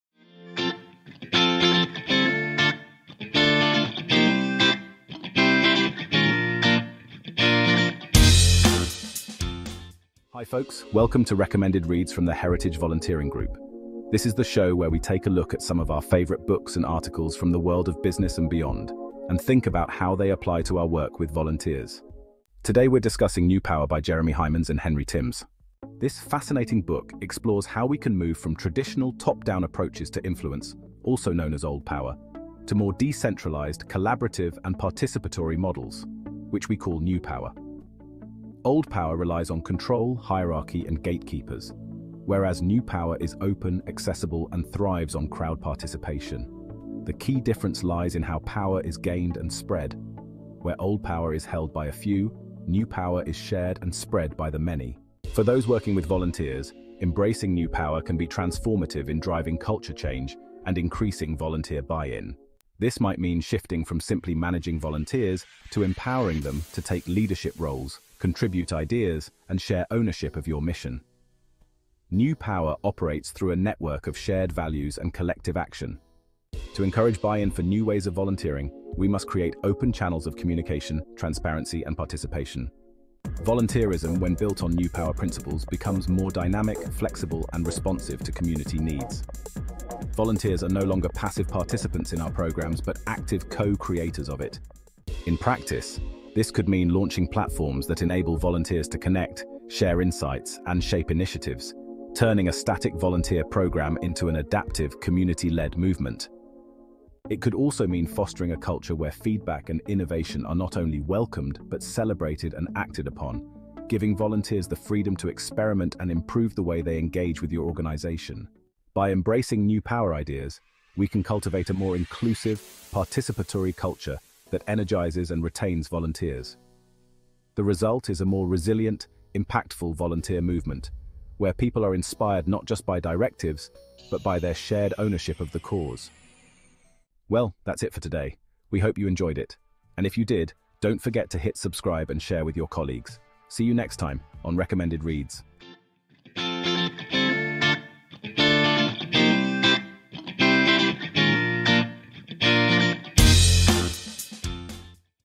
Recommended Reads: Unlocking the Power of Volunteering
In this Recommended Read we explore how New Power can help volunteer engagement professionals create deeper impact through volunteering.

To learn more about the Heritage Volunteering Group visit: heritagevolunteeringgroup.org.uk/